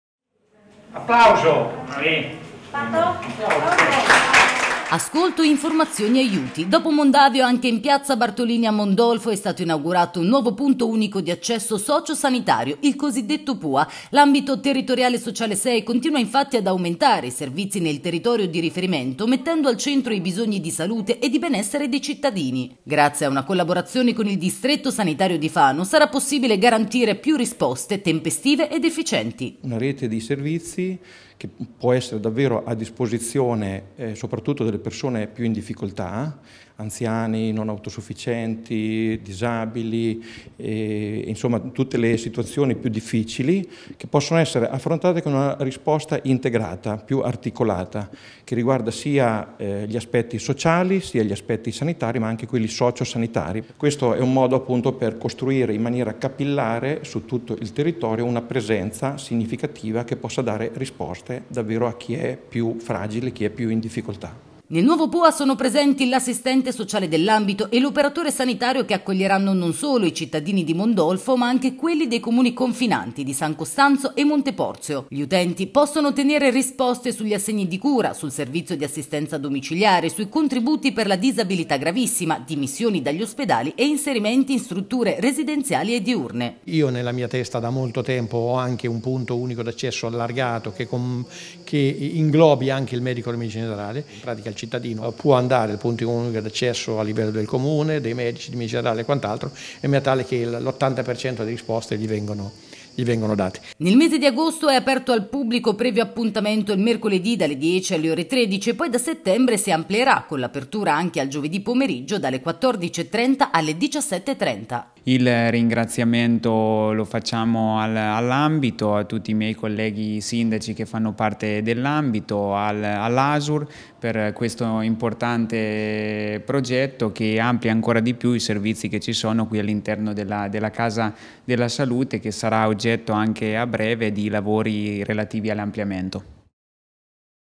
Mondolfo, punto unico di accesso socio-sanitario
A Mondolfo è stato inaugurato un nuovo Punto Unico di Accesso socio-sanitario.